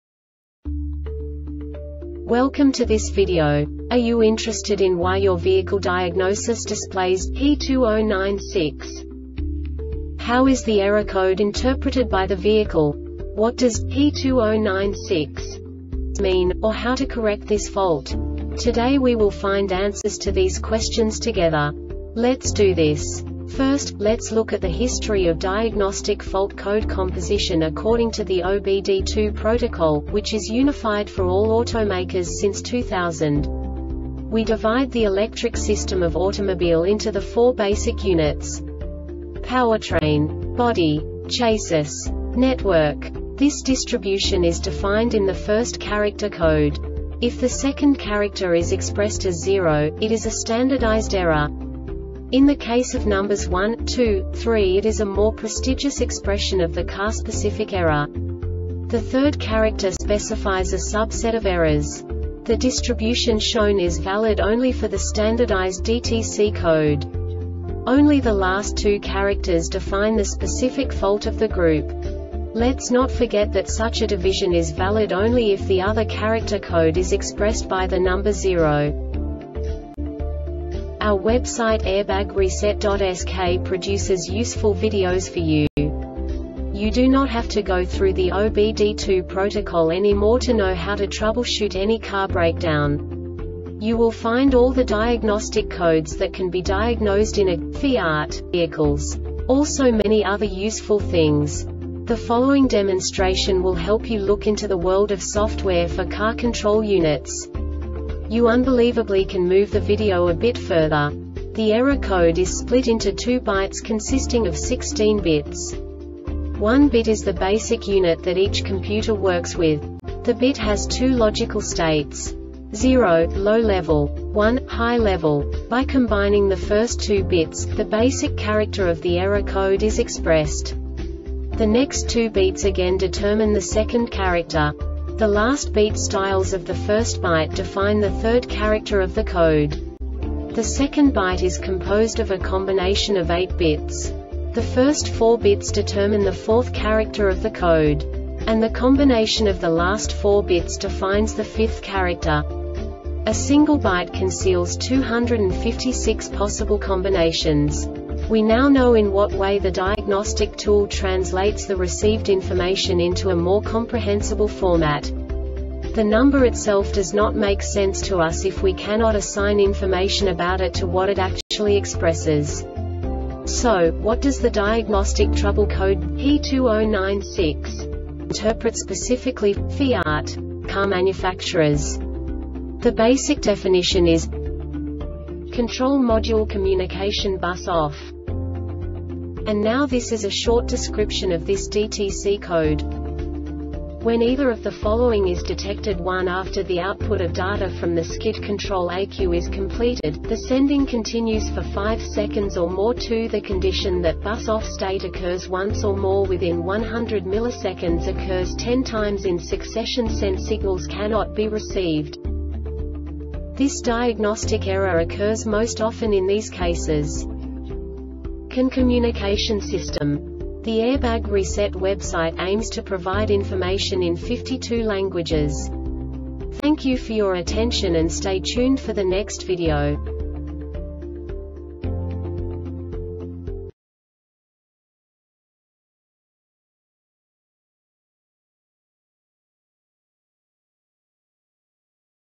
DTC Fiat P2096 Short Explanation
Contents:
0:21 Basic DTC analysis according to OBD2 protocol standard.
1:48 Insight into programming
2:58 A diagnostically understandable description of the Diagnostic Trouble Code
3:05 Basic error definition

Make:
FIAT
Code:
P2096
Definition:
Control Module Communication BUS Off
Description:
When either of the following is detected: 1. After the output of data from the skid control ECU is completed,the sending continues for 5 seconds or more.2. The condition that bus OFF state occurs once or more within 100 ms occurs 10 times in succession.(Sent signals cannot be received. )
Cause: